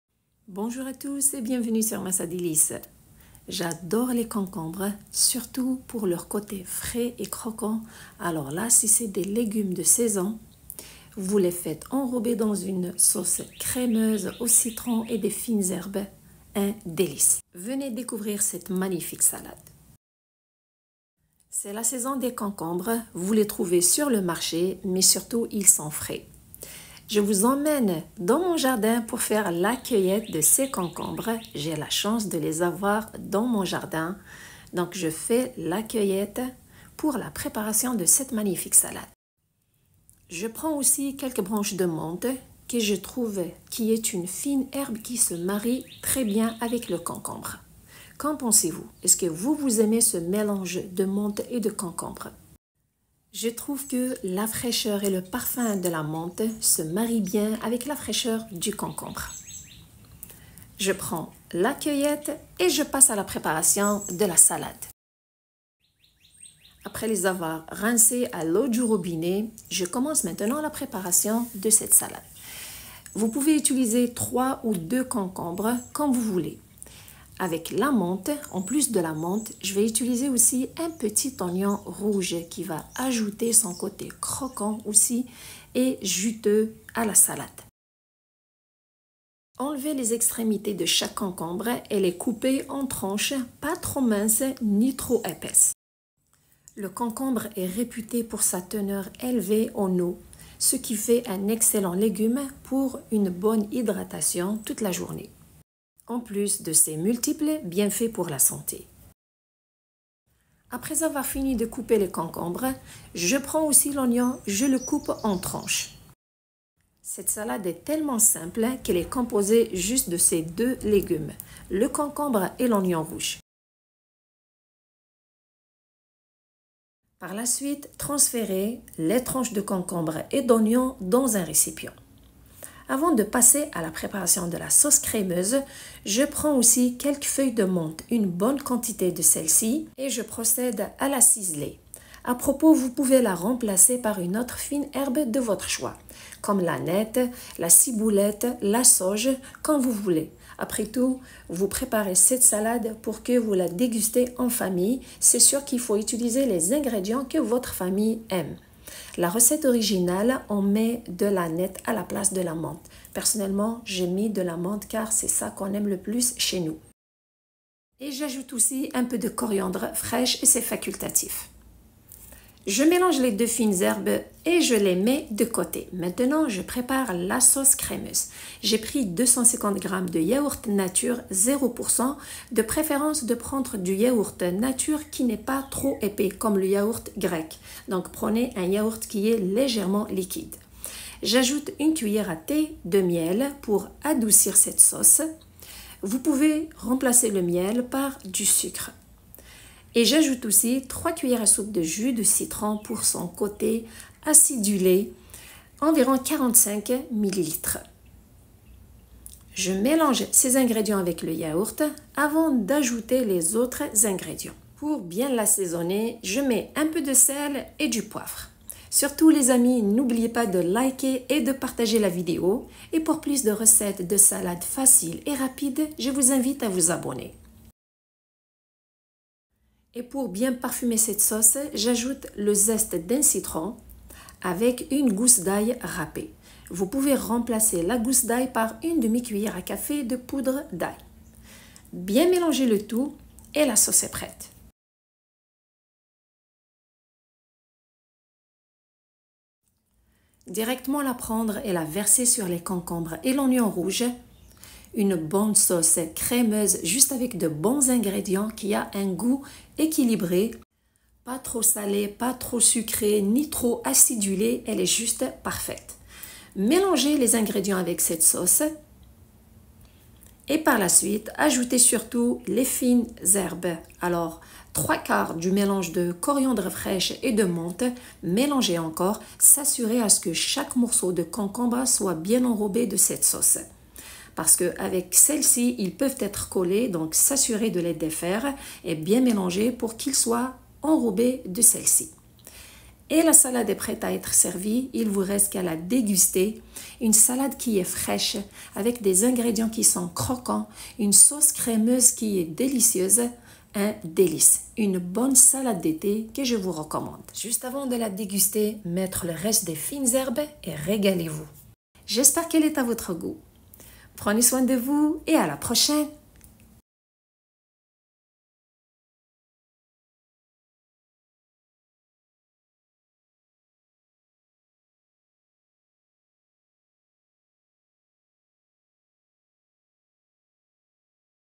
Ma Salade De Concombres Crémeuse, Facile, Simple, Croquante et Rafraîchissante, Parfaite pour L’été.
J’adore les concombres pour leur coté frais et croquant. Surtout s’ils sont enrobés d’une sauce crémeuse avec des fines herbes et du citron 🍋.
Un régal que je vous invite à découvrir.